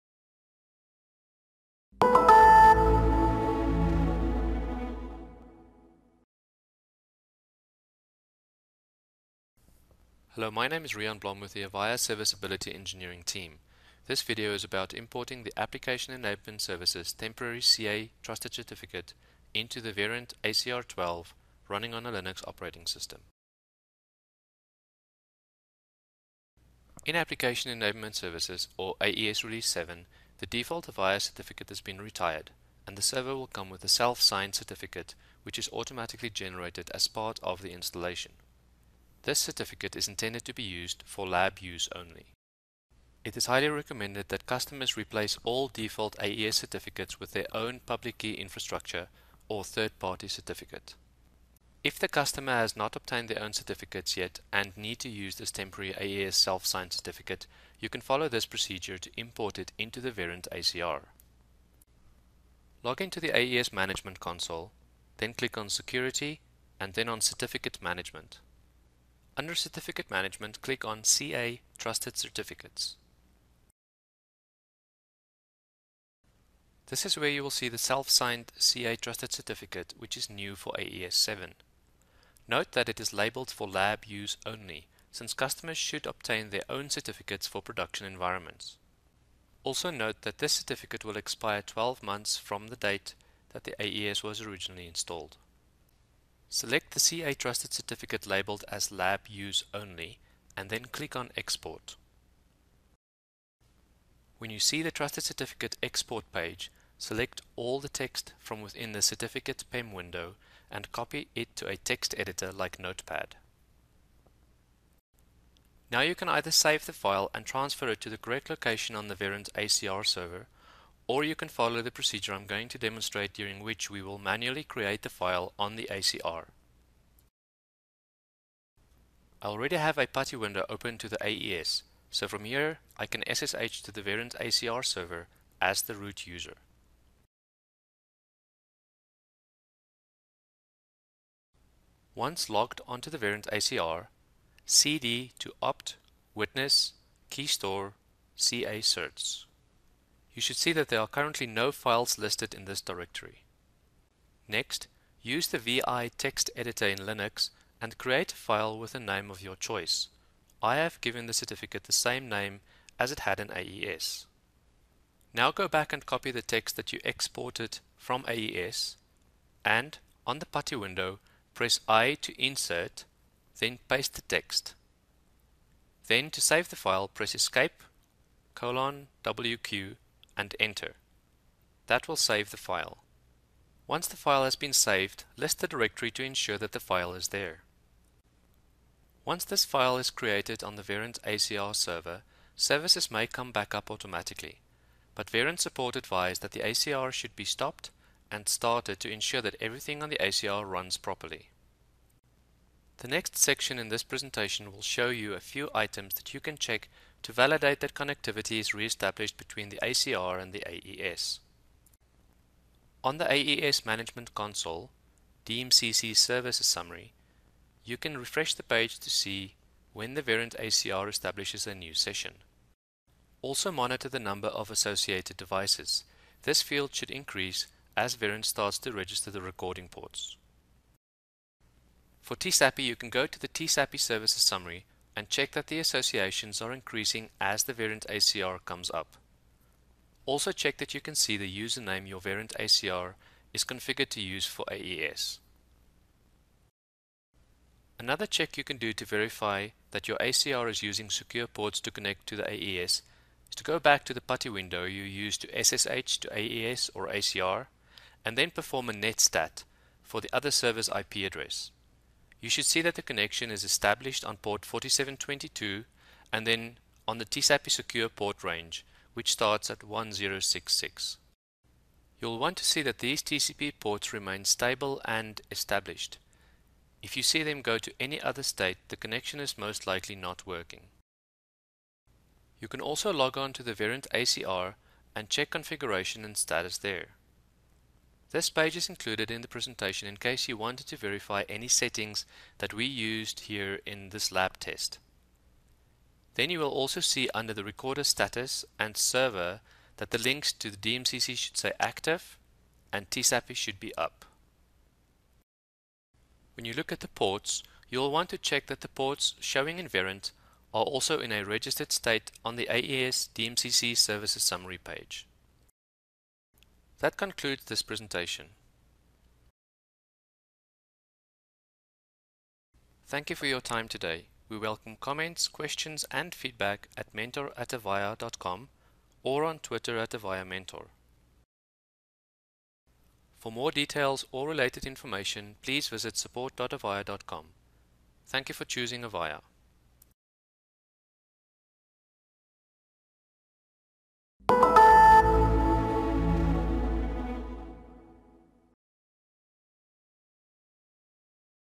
Importing AES temporary CA Trusted Certificate into Verint ACR 12 on Linux
This Video demonstrates the procedure of Importing AES 7 temporary CA Trusted Certificate into Verint ACR 12 on Linux.

Produced by: Riaan Blom